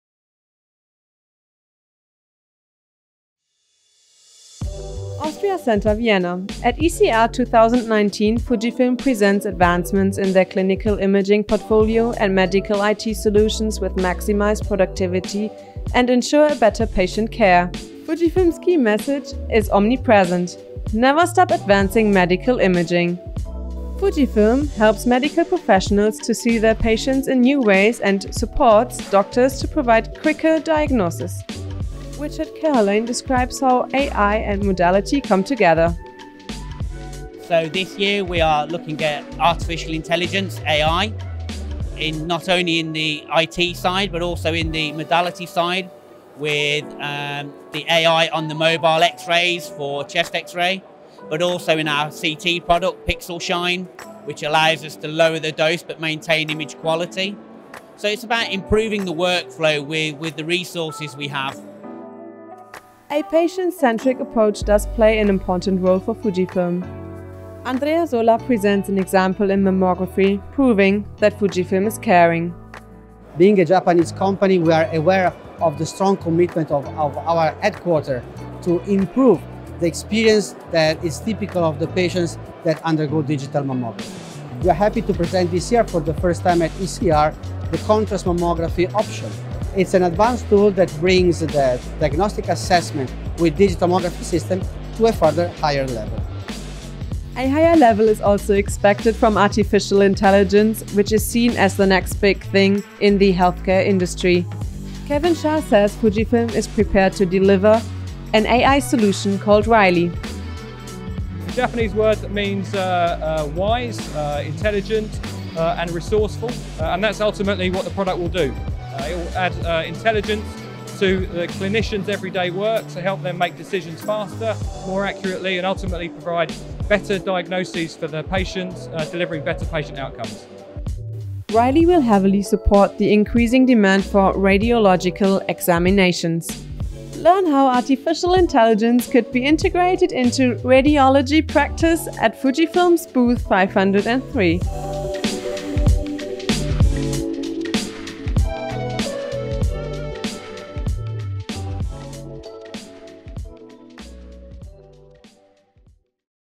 Fujifilm presents AI initiative REiLI at ECR 2019
The European Congress of Radiology (ECR) is the annual meeting of the European Society of Radiology (ESR). Fujifilm presents its whole radiological portfolio as well as REiLI, Fujifilm’s AI technology initiative at this trend-setting, dynamic and service-oriented congress.

Filmed on FUJIFILM X-T3.